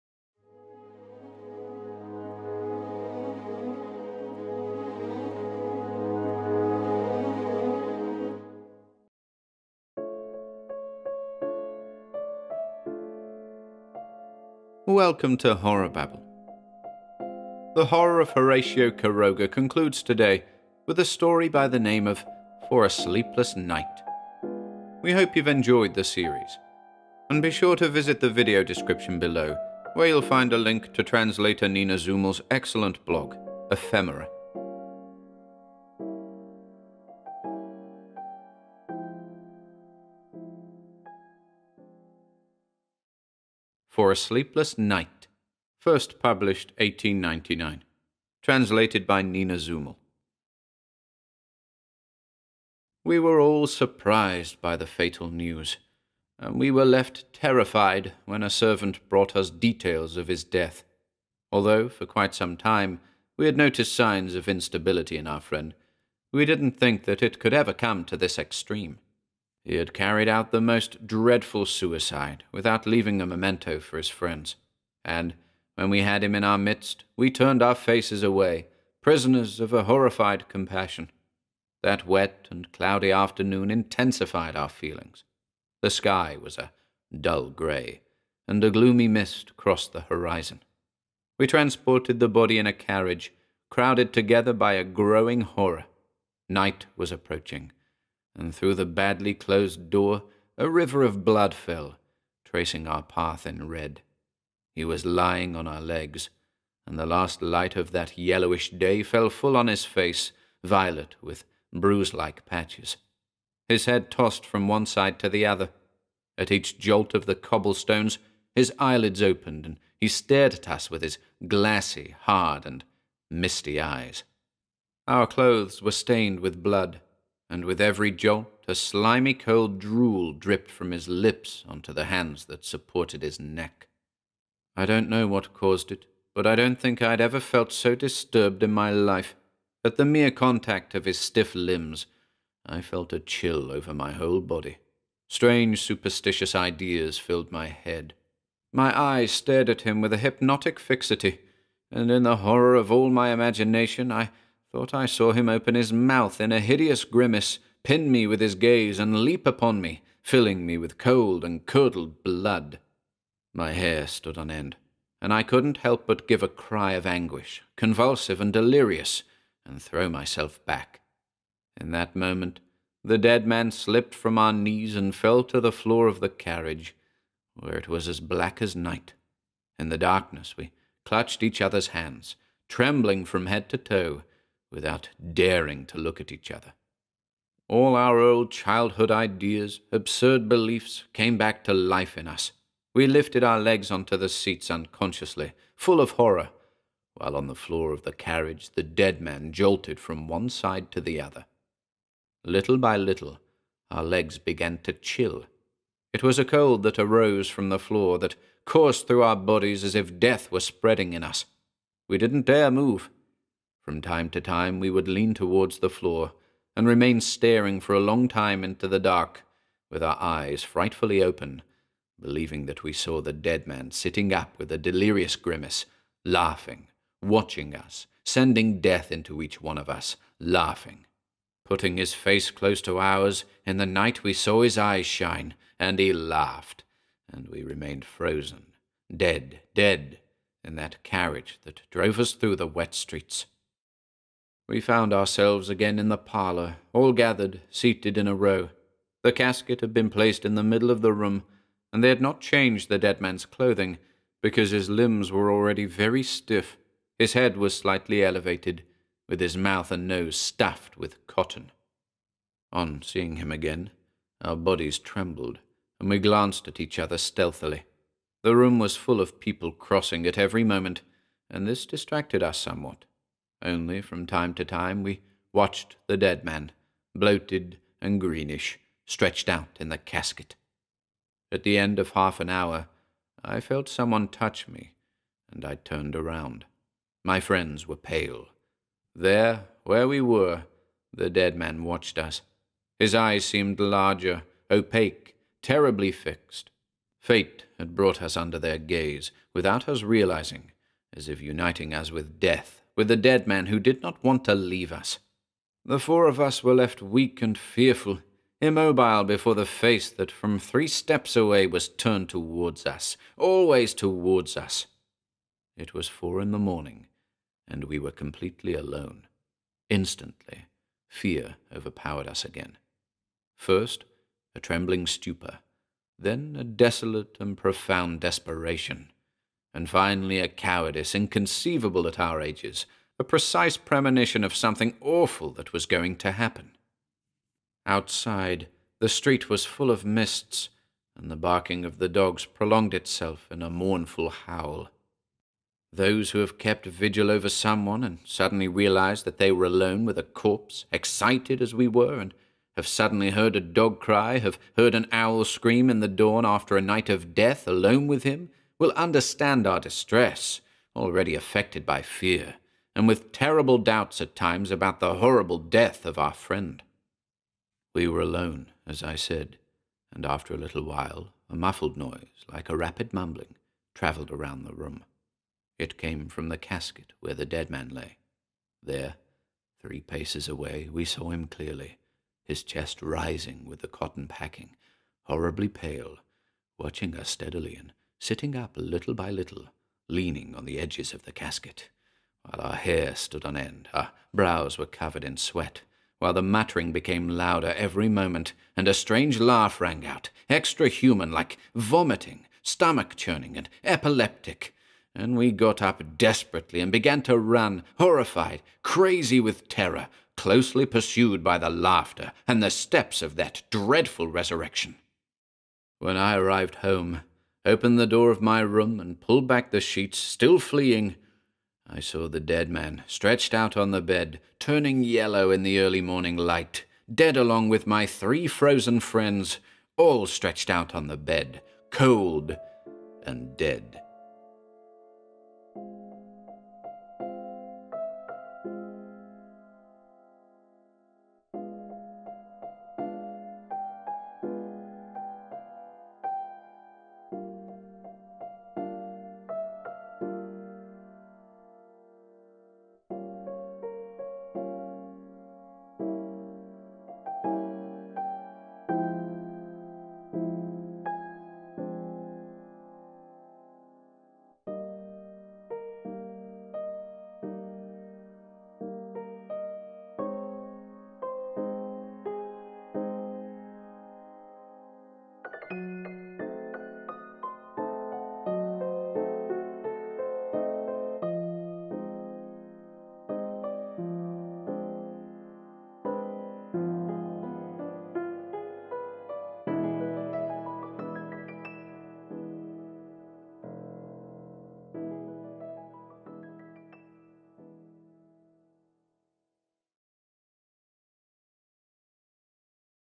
"For a Sleepless Night" / The Horror of Horacio Quiroga
The Horror of Horacio Quiroga
Episode 5: For a Sleepless Night

00:00 - Introduction
00:42 - For a Sleepless Night

Horacio Quiroga was a Uruguayan author, born in Salto in 1878. As a follower of modernism, and an admirer of the works of Edgar Allan Poe and Guy de Maupassant, Quiroga was compelled to author works addressing the nature of man and beast, often through use of the supernatural and the bizarre.

The stories comprising this series were brought to our attention and translated by Nina Zumel

Narrated by Ian Gordon for HorrorBabble
Music and production by Ian Gordon & Jennifer Gill

Search HORRORBABBLE to find us on:
AUDIBLE / ITUNES / SPOTIFY

This is an ORIGINAL HorrorBabble Production.